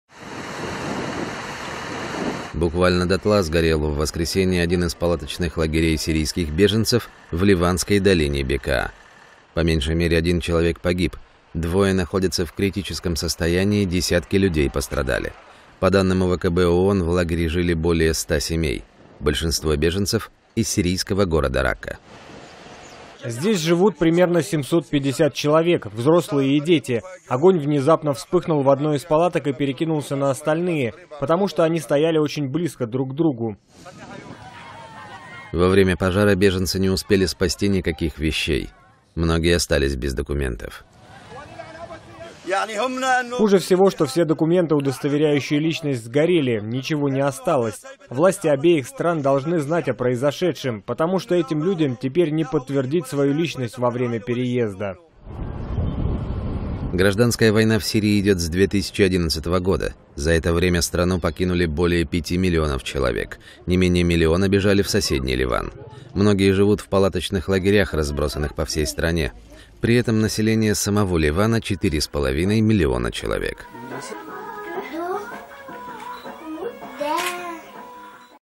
В Ливане сгорел лагерь сирийских беженцев, есть погибший (новости)
В Ливане сгорел лагерь сирийских беженцев, есть погибший. Буквально дотла сгорел в воскресенье один из палаточных лагерей сирийских беженцев в ливанской долине Бекаа. По меньшей мере, один человек погиб, двое находятся в критическом состоянии, десятки людей пострадали.

По данным УВКБ ООН, в лагере жили более 100 семей. Большинство беженцев – из сирийского города Ракка.

[сирийский беженец]:
«Здесь (живут) примерно 750 человек: взрослые и дети. Огонь внезапно вспыхнул в одной из палаток и перекинулся на остальные, потому что они стояли очень близко друг к другу».

Во время пожара беженцы не успели спасти никаких вещей. Многие остались без документов.

[сирийский беженец]:
«Хуже всего, что все документы, удостоверяющие личность, сгорели, ничего не осталось. Власти обеих стран должны знать о произошедшем, потому что этим людям теперь не подтвердить свою личность во время переезда».

Гражданская война в Сирии идёт с 2011 года. За это время страну покинули более пяти миллионов человек. Не менее миллиона бежали в соседний Ливан. Многие живут в палаточных лагерях, разбросанных по всей стране. При этом население самого Ливана – четыре с половиной миллиона человек.